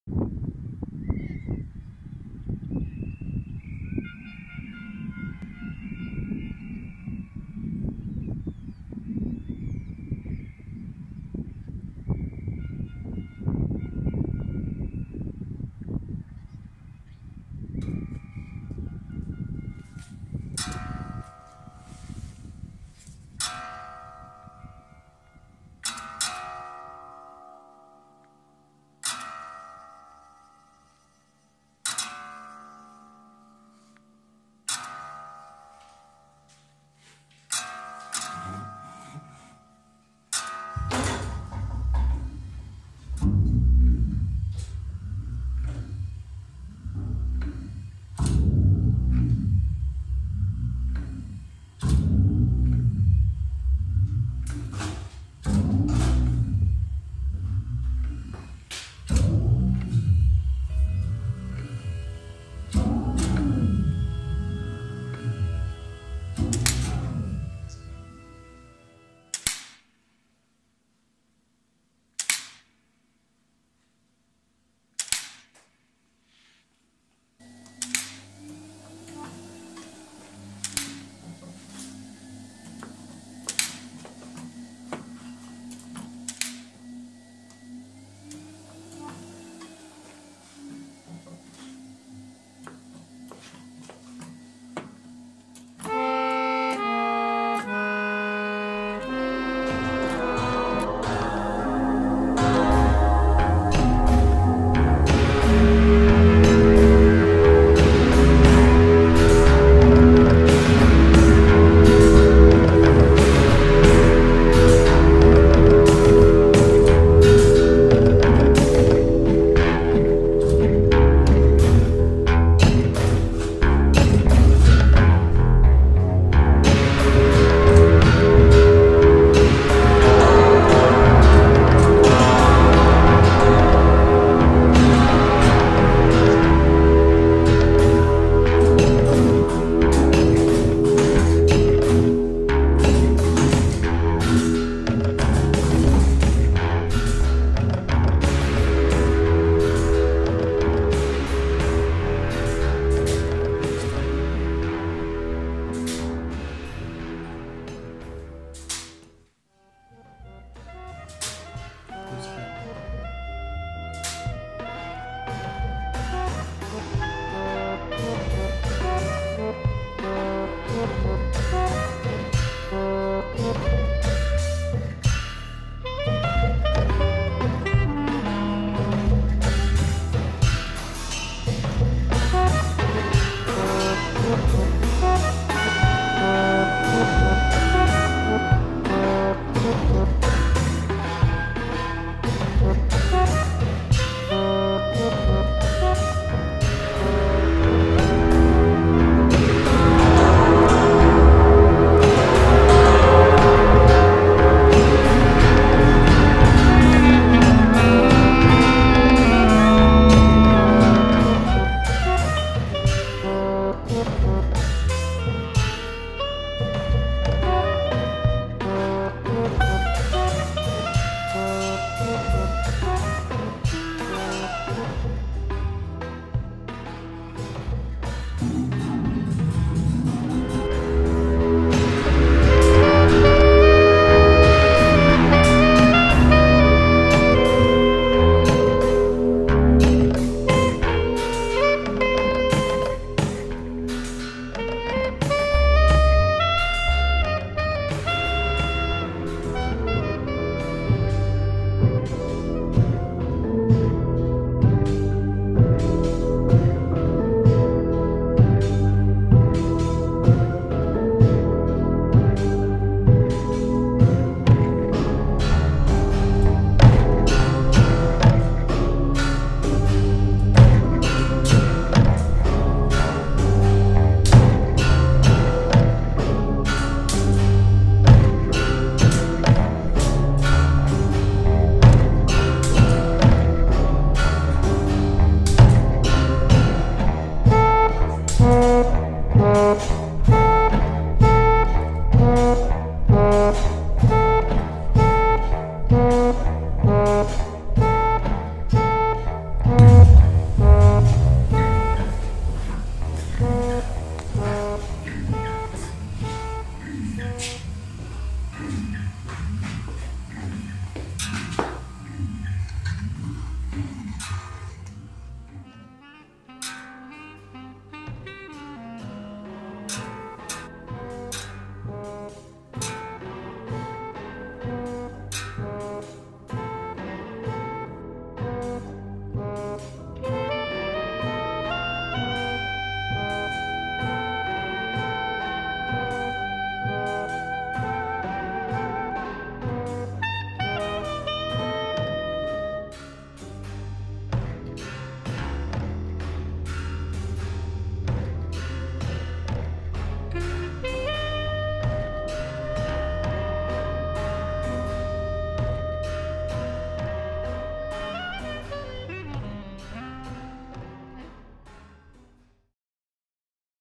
In progress pt1, the recordings of the Mechanical Orchestra
In the summer of 2021, Perned & Stålhammar began an experimental collaboration with the artist and sculptor Jan Cardell and his Mechanical Orchestra. The Mechanical Orchestra is today a magnificent collection of sculptures that have emerged under Cardell's hands since the late 90's. Today it consists of more than 200 sculptures.

Soon we all understood the potential of reconstructing the magnificent sculptures into musical instruments. An avalanche of challenges where the aesthetics of metal and wood are offered to meet the music's fine calibration in a rustic fusion and detailed study in tones and harmony.

This is the first phase.